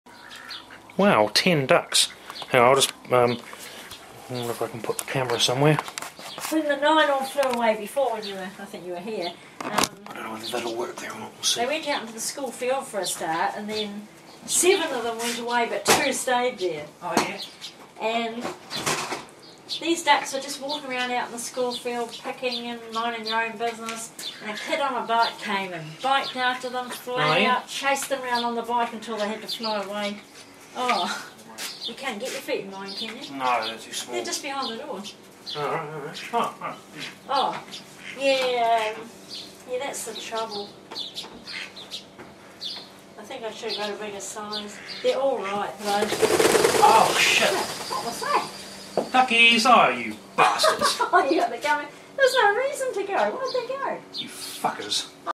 Ten Ducks Fly Away
One day, TEN ducks landed on our back lawn all at the same time - and they all flew away at the same time.